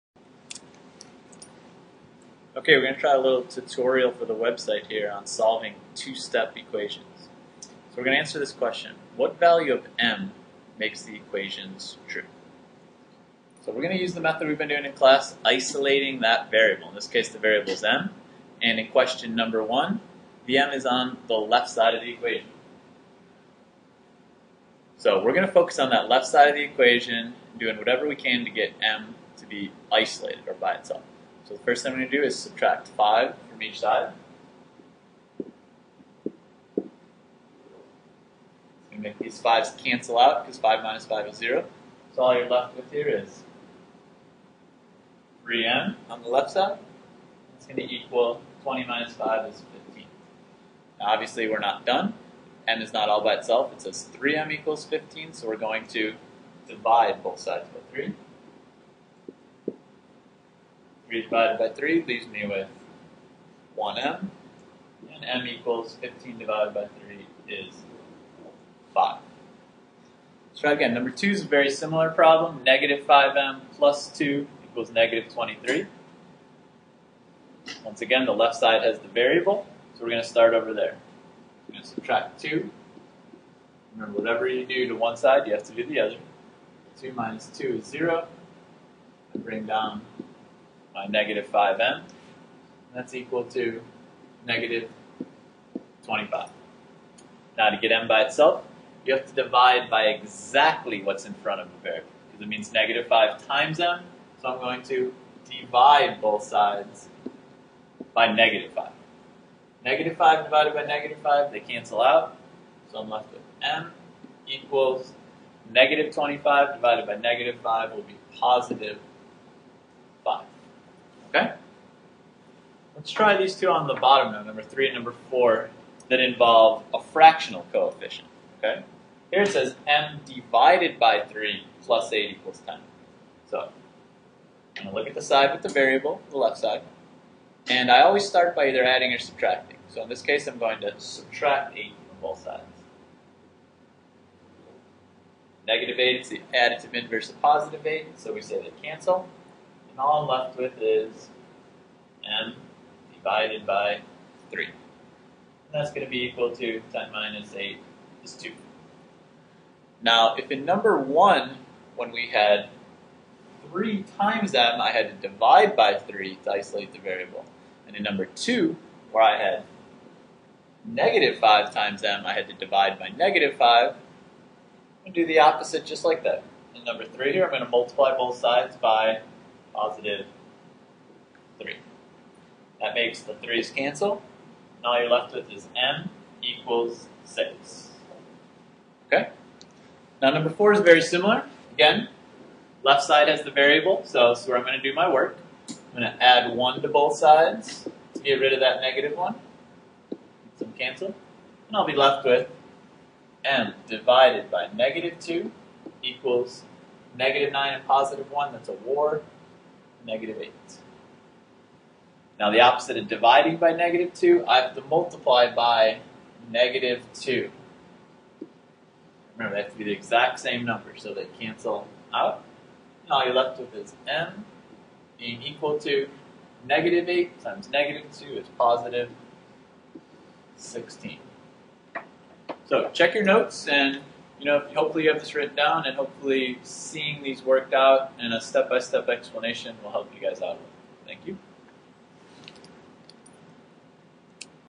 2Step Equations
How to solve 2step equations.